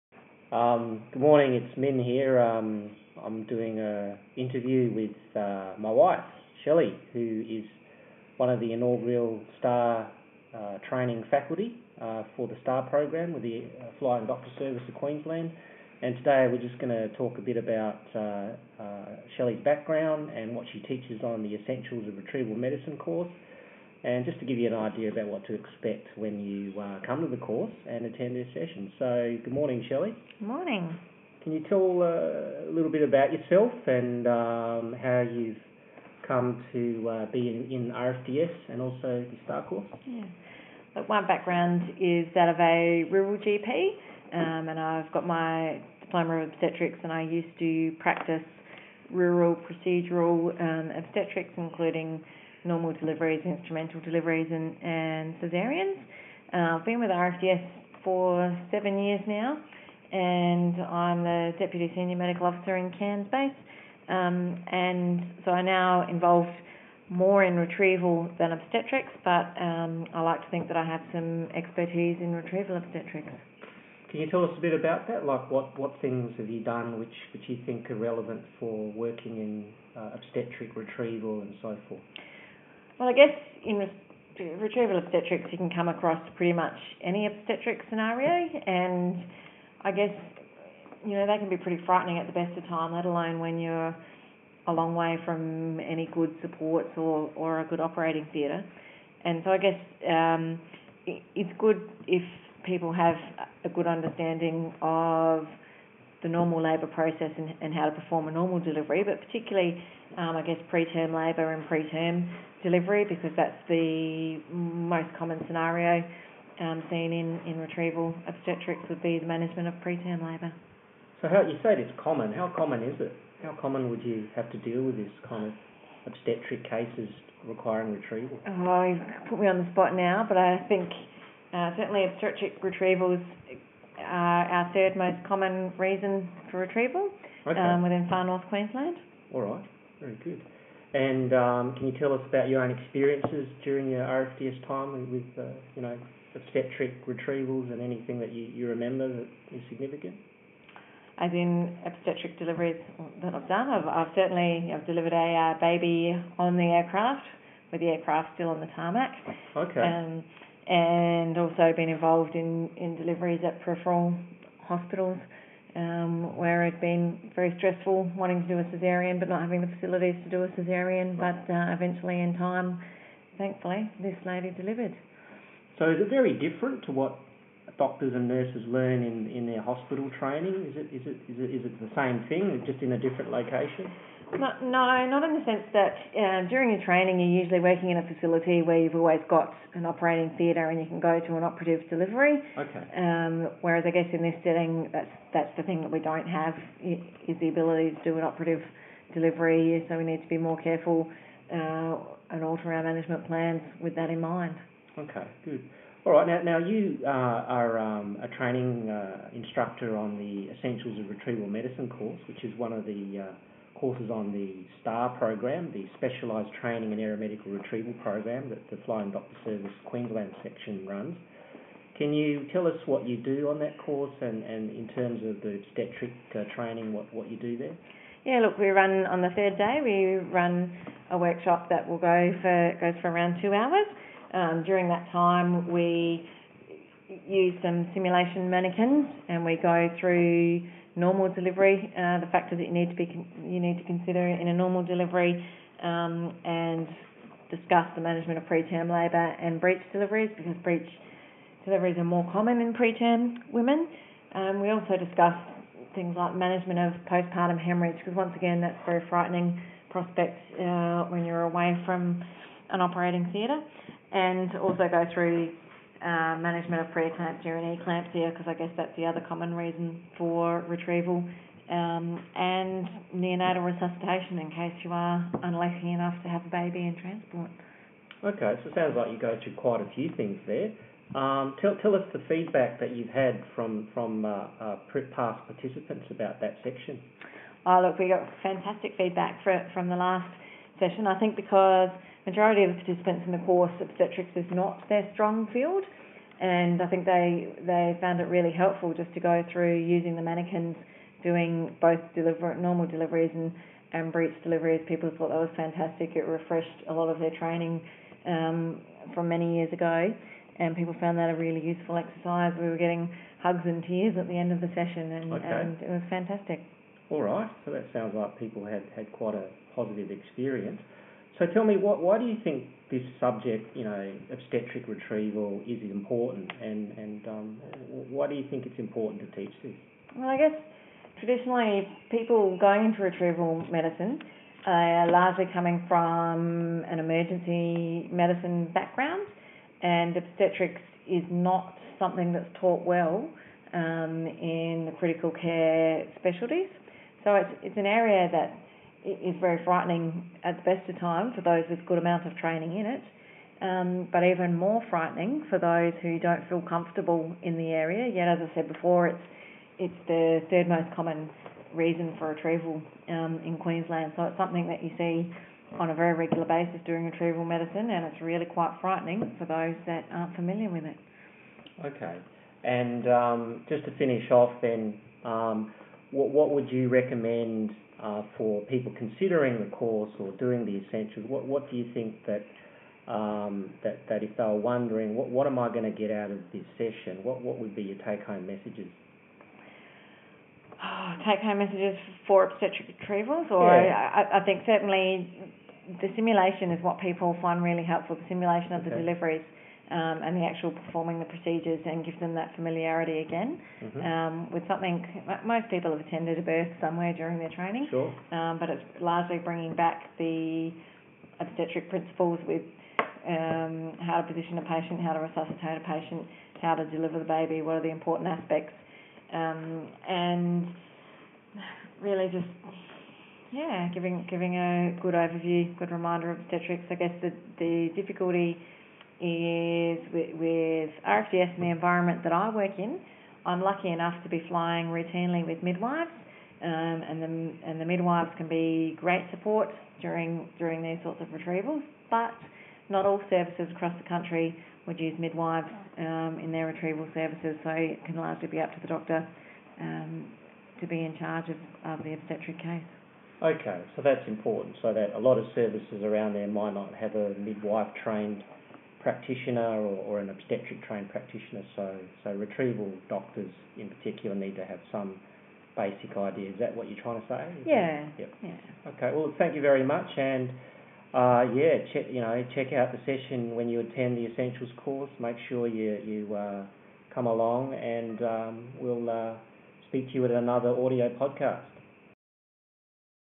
STAR Faculty - Shelley LeCong - Obstetric Retrieval
STAR Faculty member Dr Shelley LeCong talks about Obstetric Retrievals and the STAR Course